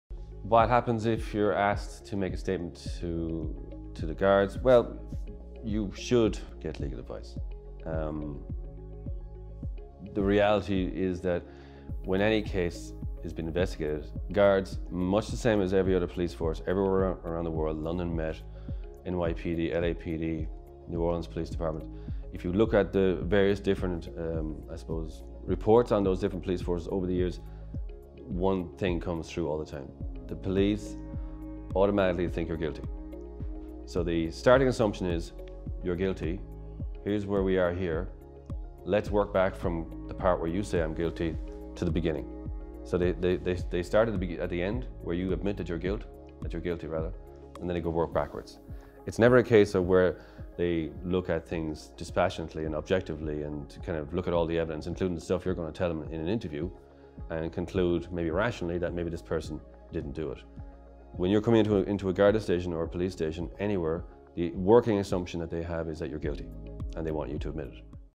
Do you have to give a statement to Gardai?
No. But sometimes it makes sense to do just that.

The first words of the legal caution are as follows: "You are not obliged to say anything unless you wish to do so..." so making a statement is not mandatory under law.

The right to silence prevails in all constitutional democracies in the West and Ireland is no different.

However merely telling a Garda that you do not wish to make a statement does not mean that you cannot be arrested and brought to a Garda Station and interviewed. You can if the offence under investigation is an arrestable offence.
In that case the Gardai can arrest you and bring you to the Garda Station to be interviewed. That process doesn't change your right to silence; you may still decide to say nothing if you wish.

However in some circumstances it does make sense to make a statement. If the allegations against you are false and/or frivolous or vexatious then it makes sense to at least vehemently deny them.

After all, the vehemence of your denials is something that may impress a jury if the case goes to trial and the video recording of your Garda interview is played to them.